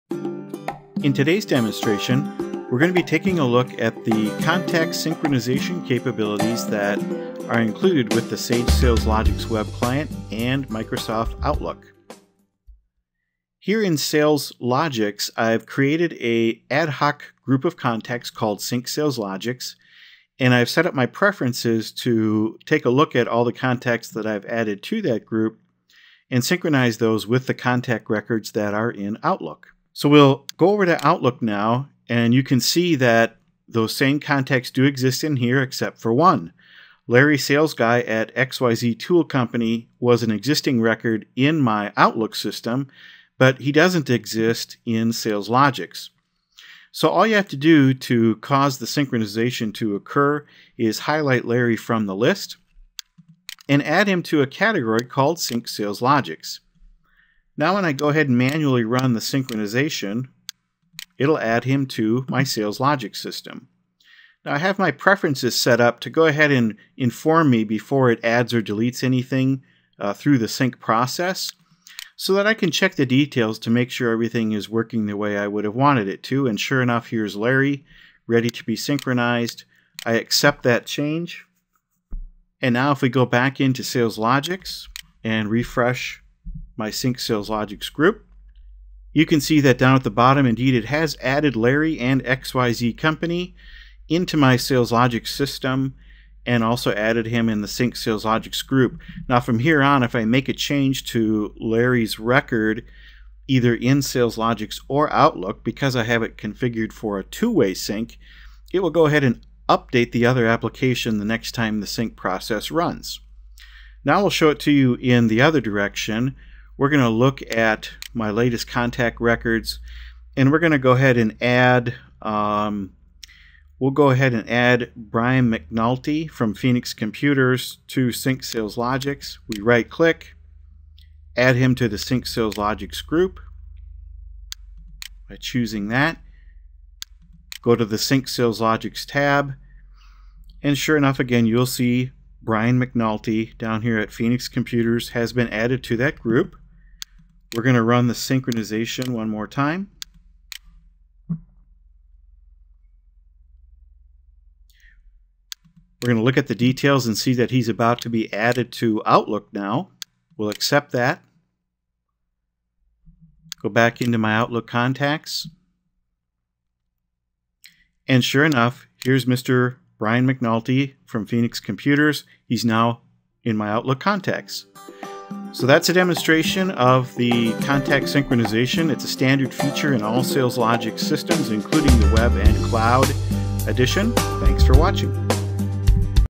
SalesLogix Outlook Contacts Sync - SalesLogix Demo Series
- This video demonstrates the capability of syncronizing SalesLogix and Outlook Contacts